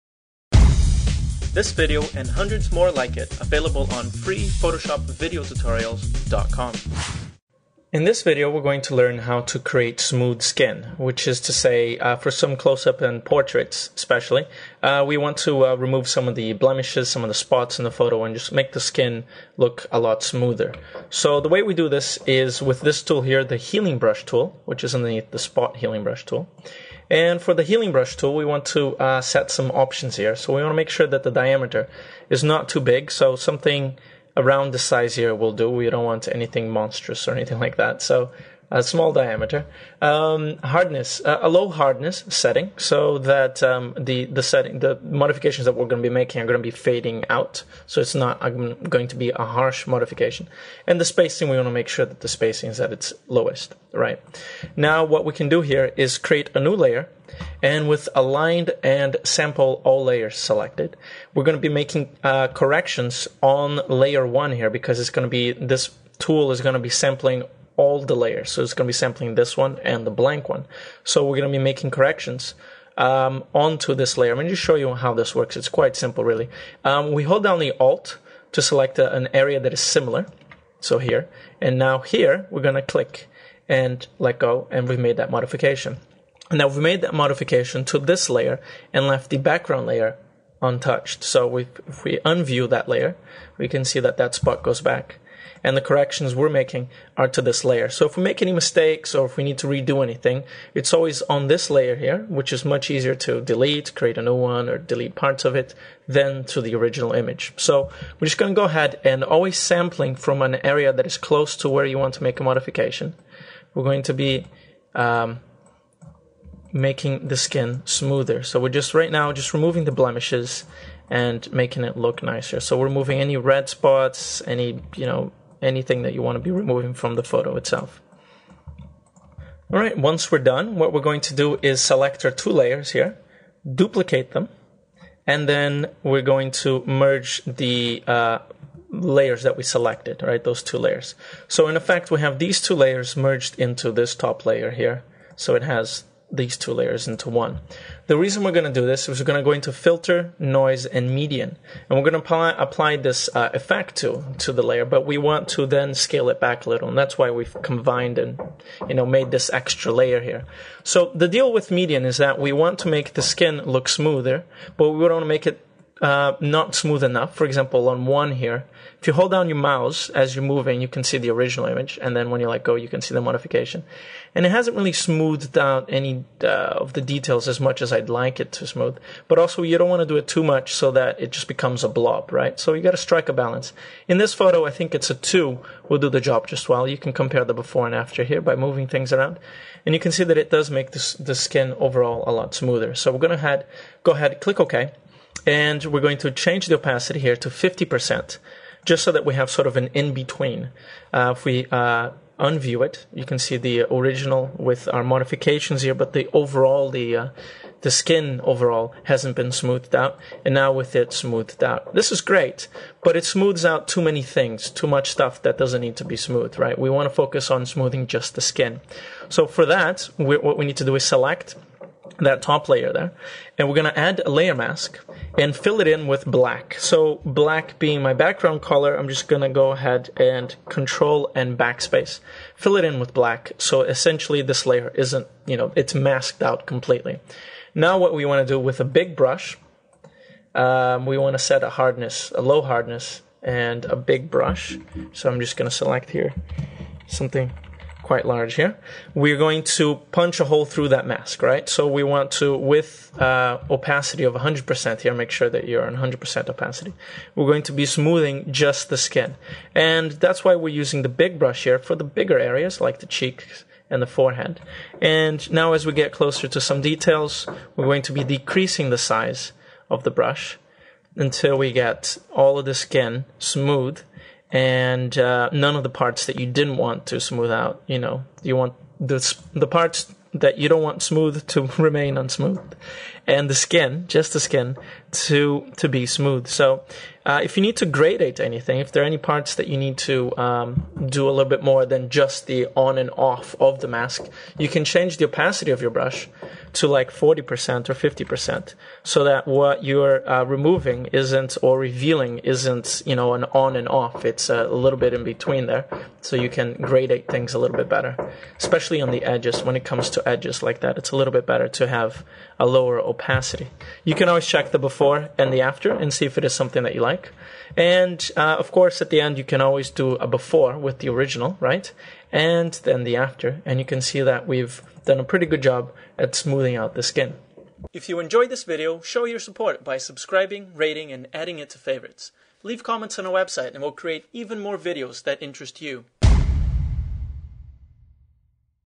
Learn Photoshop - How to Add Smooth Skin to Portraits
Learn how to make skin look smooth, great for portraits and close ups! Here we cover the use of the healing brush tool, median filter, and a layer mask to achieve the desired look.